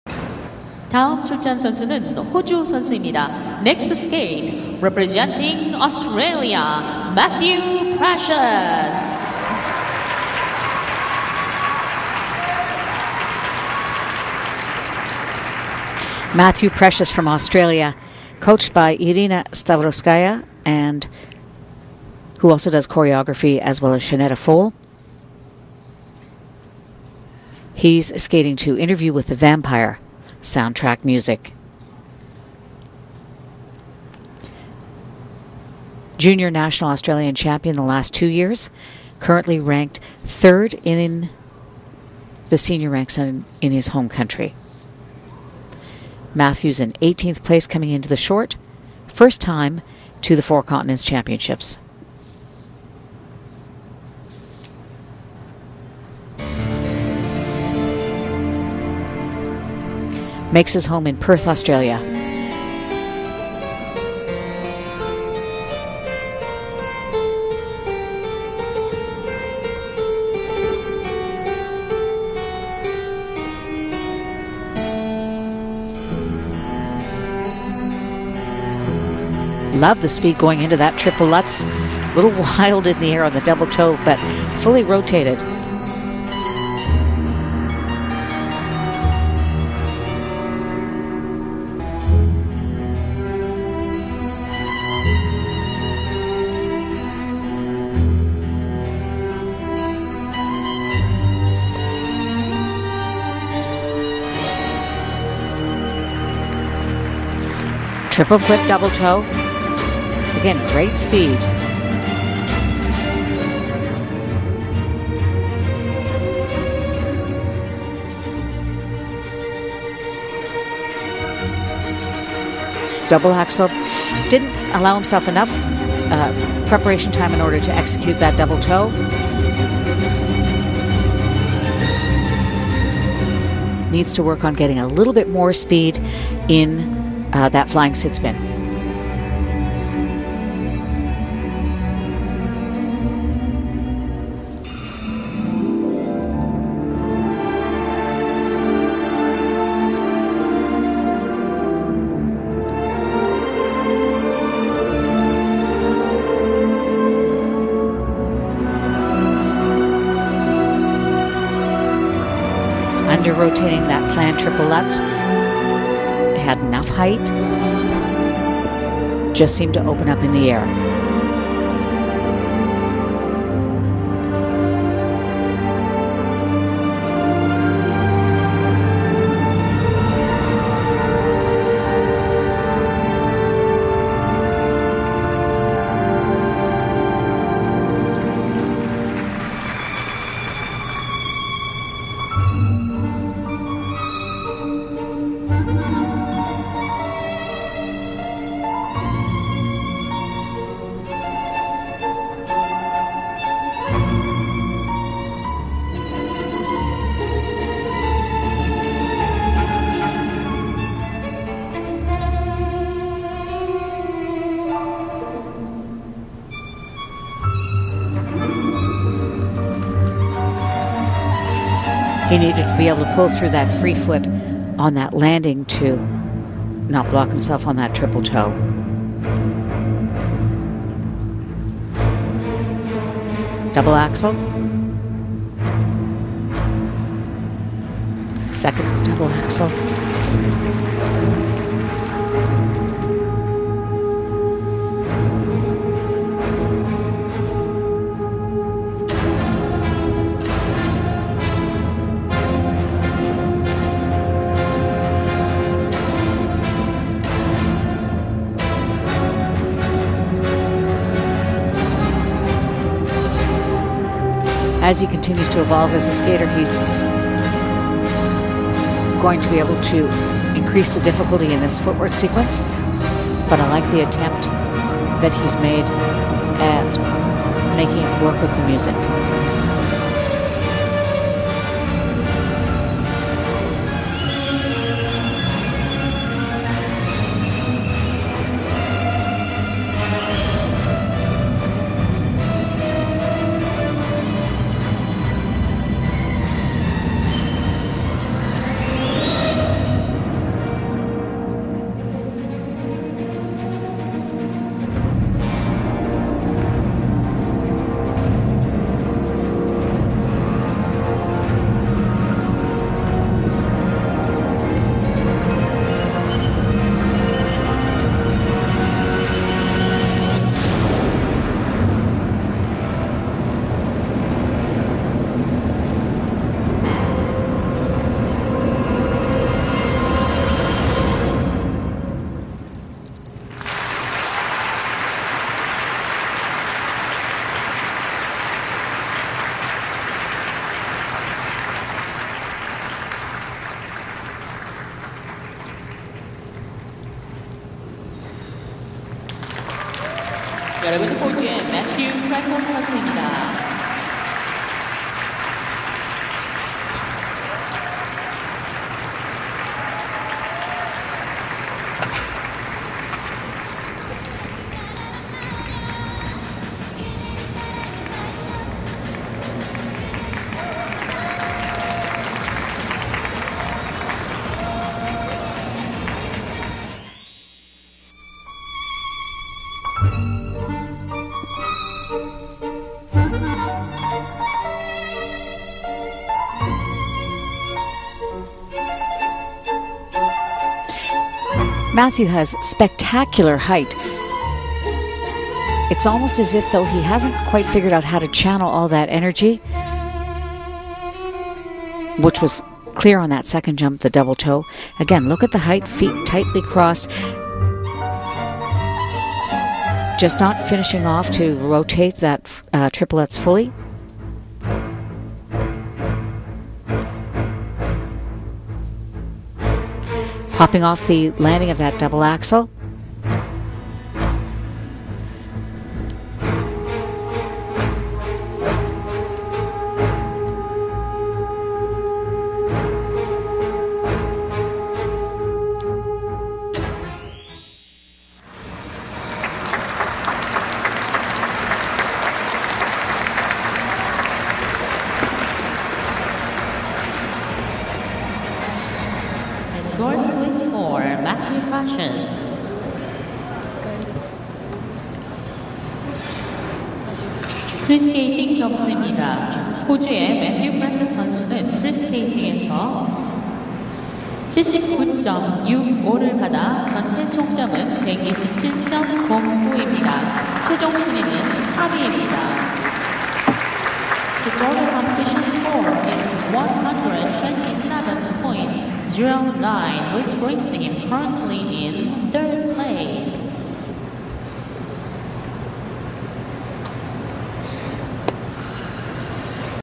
Matthew Precious FP
Matthew Precious skates his free program at the 2010 Four Continents Championships in Korea